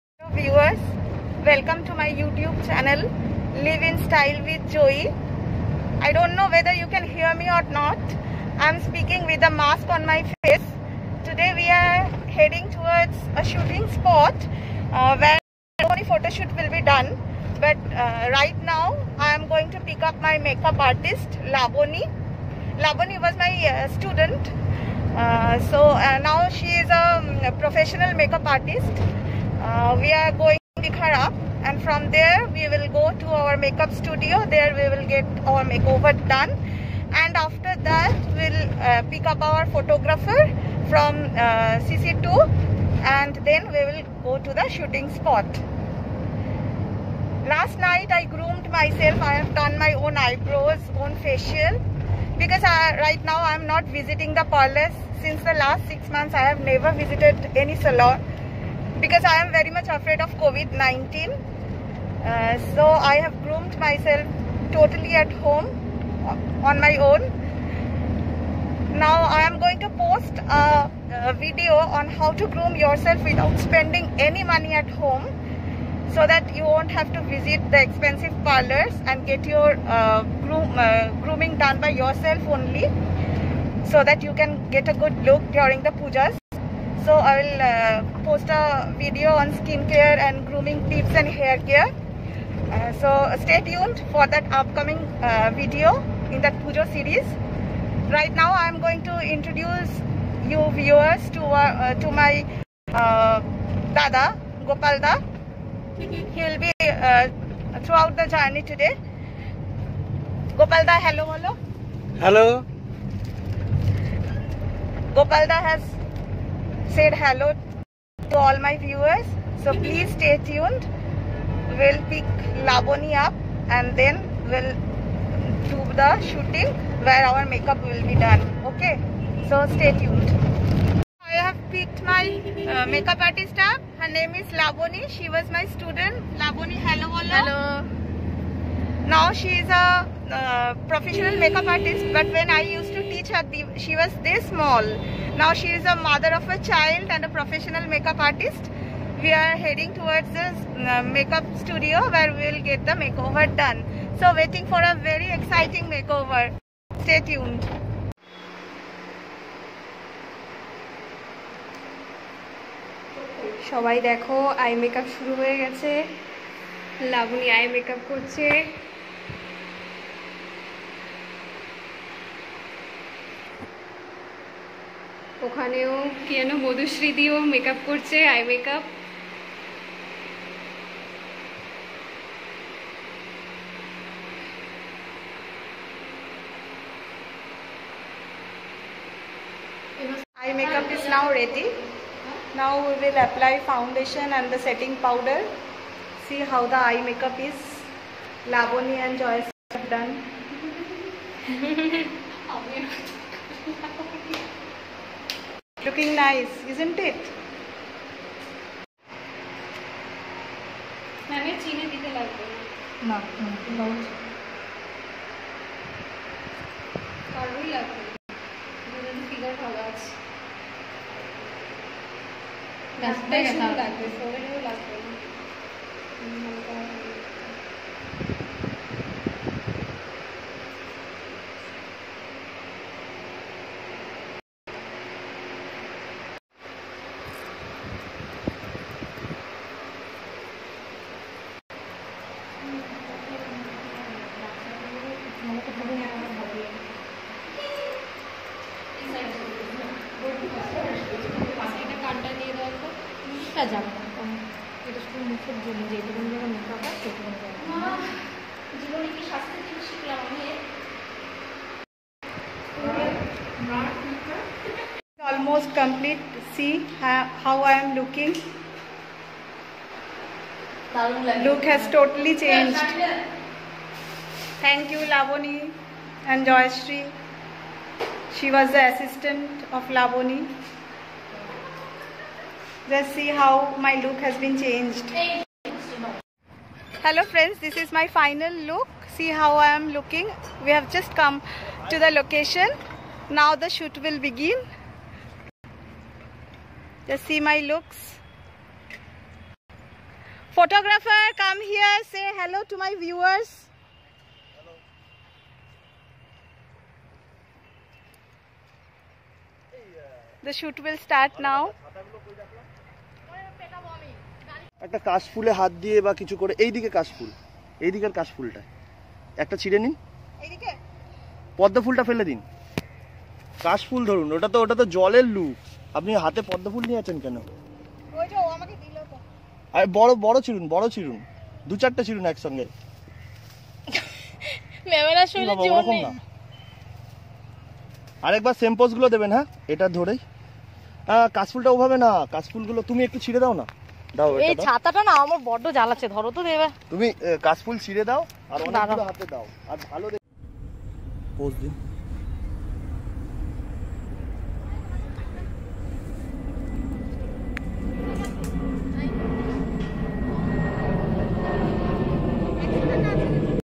Agamani Photoshoot...Behind the Scenes Vlog#behindthescenes
A new video is coming up on the auspicious occasion  of Mahalaya...Agamani Photoshoot.This is the... Behind the Agamani Photoshoot Vlog...I have started my own  youtube channel only 2 weeks ago.Learning the editing on my own by trial nd error method.I am not a pro...so please forgive my flaws nd watch the video.